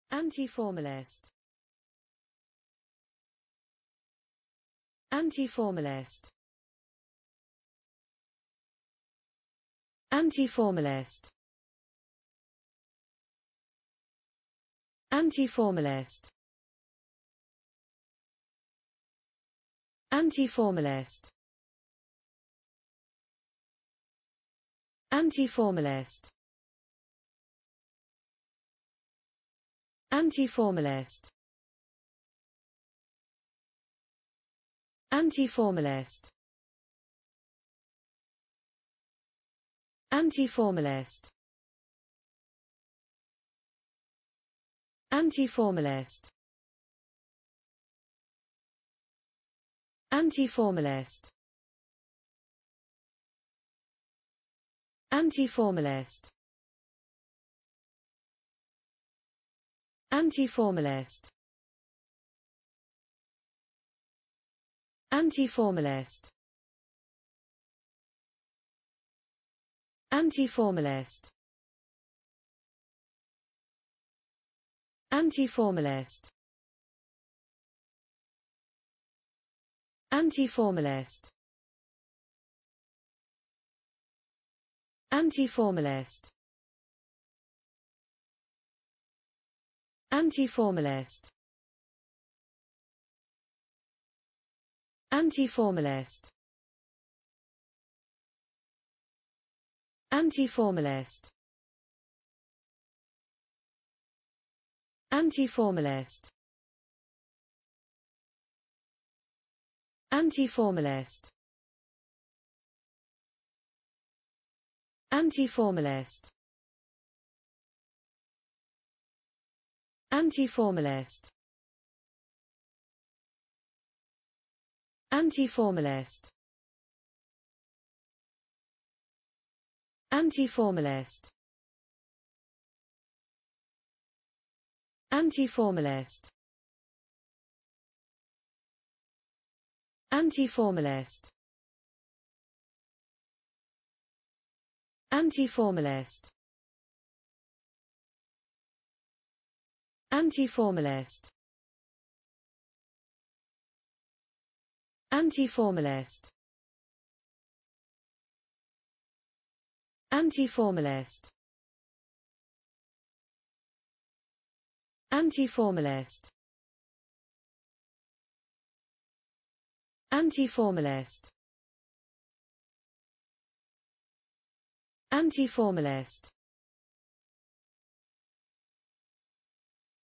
Anti-formalist: How to pronounce Anti-formalist in english (correct!).Start with A. Learn from me.
Follow me!Learn from me how to pronounce the word Anti-formalist in english.This video shows you how to pronounce the word Anti-formalist in english.

Anti-formalist